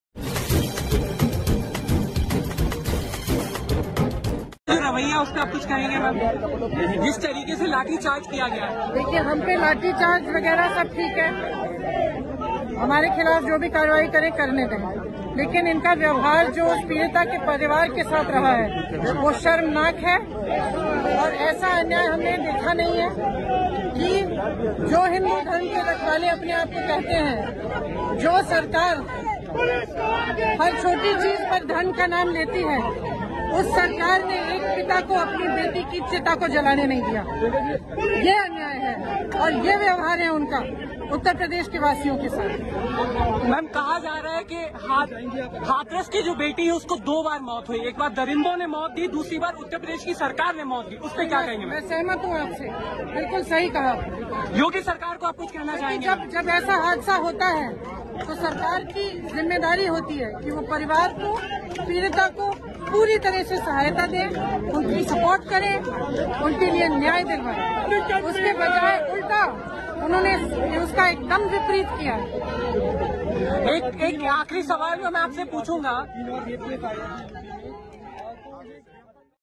போலீசாரின் லத்தி பாதிக்கப்பட்ட பெண்ணை பாதுகாக்க பயன்பட்டிருக்க வேண்டும்" - பிரியங்கா காந்தி !
qtv24x7 போலீசாரின் லத்தி பாதிக்கப்பட்ட பெண்ணை பாதுகாக்க பயன்பட்டிருக்க வேண்டும்" - பிரியங்கா காந்தி !